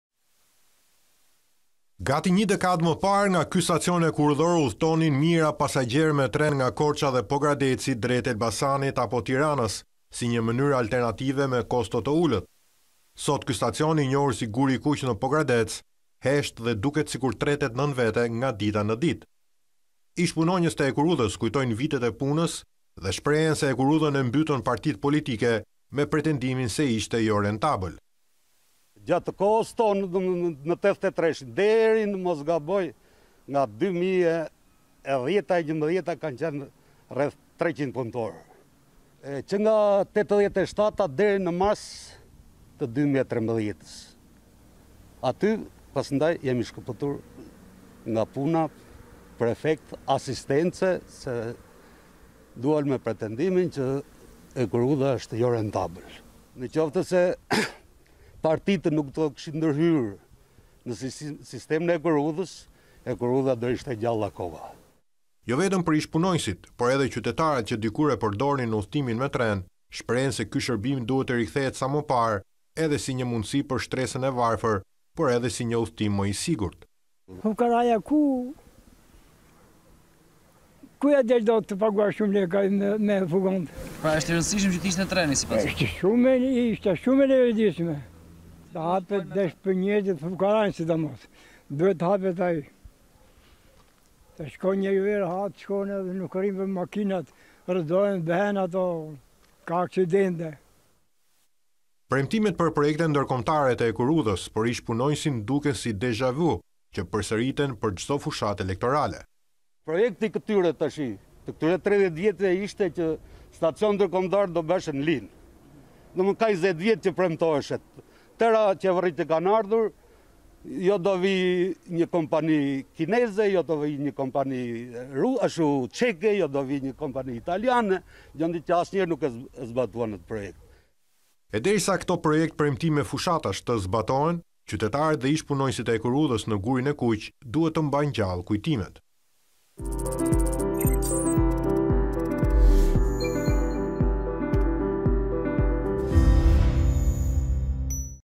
“Guri i kuq”, stacioni që dikur lidhte juglindjen
Gati një dekadë më parë, nga ky stacion hekurudhor udhëtonin mijëra pasagjerë me tren, nga Korça dhe Pogradeci drejt Elbasanit apo Tiranës, si një mënyrë alternative me kosto më të ulët.